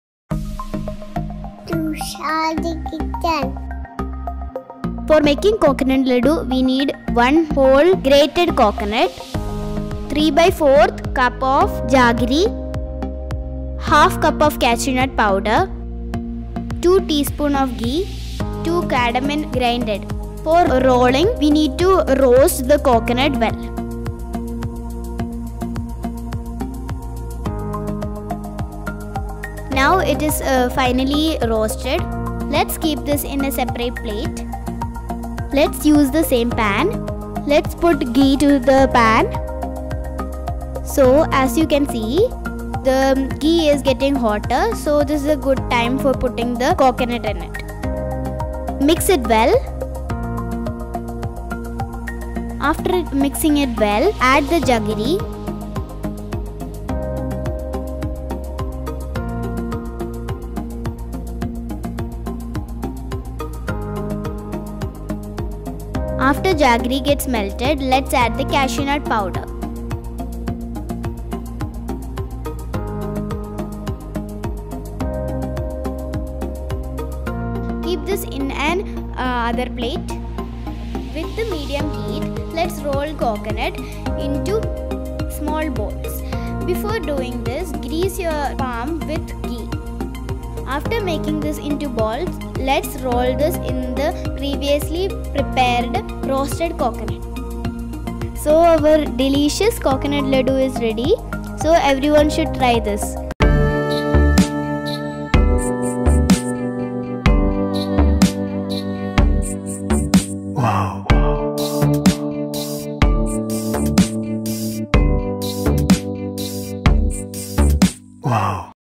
Coconut ladoo II English
Coconut ladoo are delicious sweetened balls made with coconut, sugar, cardamom powder . These are one of the most easiest of all Indian sweets recipes that can be made for any festival or occasion.